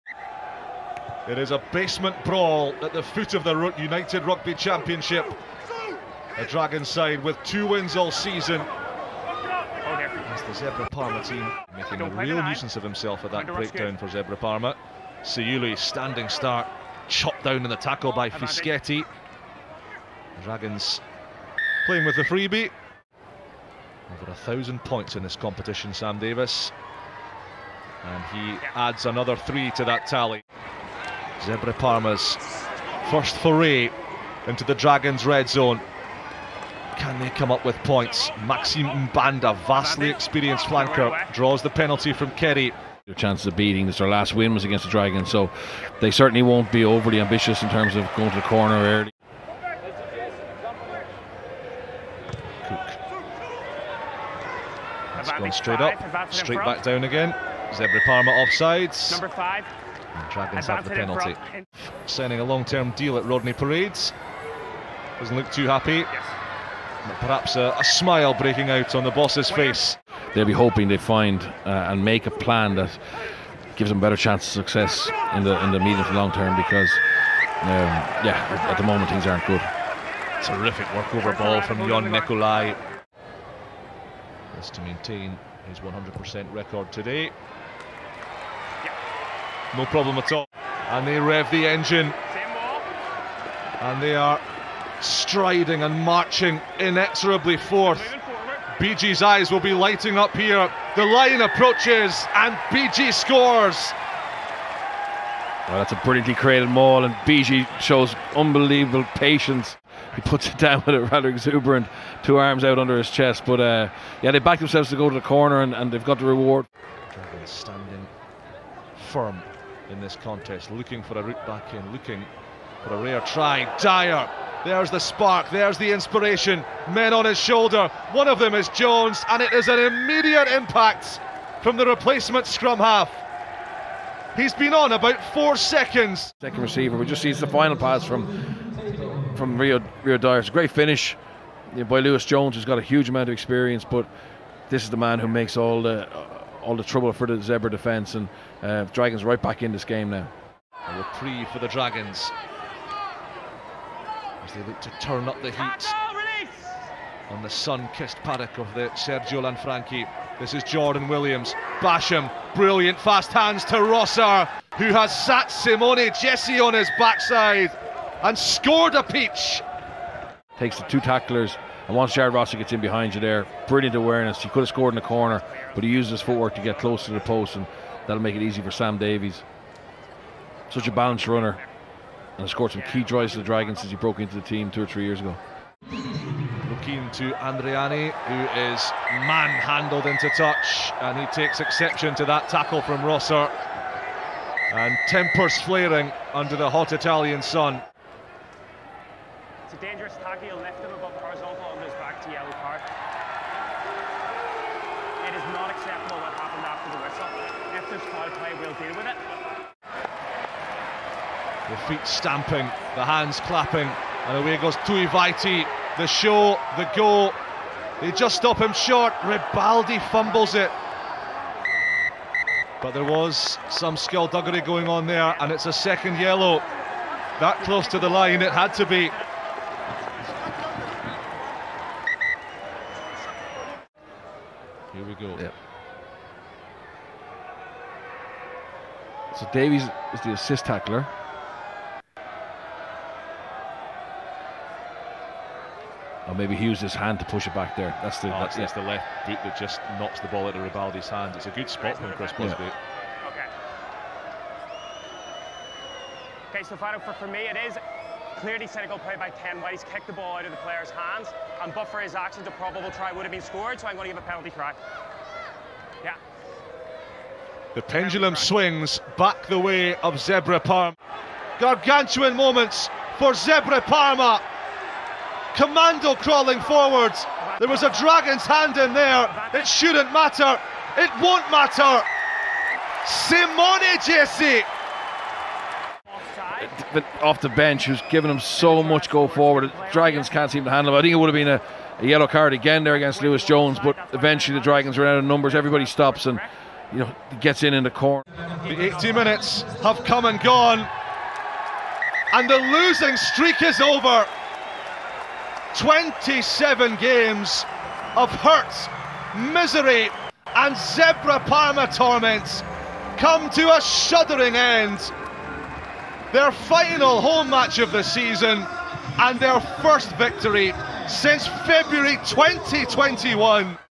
Zebre Parma vs Dragons - Highlights from URC
Watch the highlights from Zebre Parma 23-18 Dragons, URC, 30/4/2022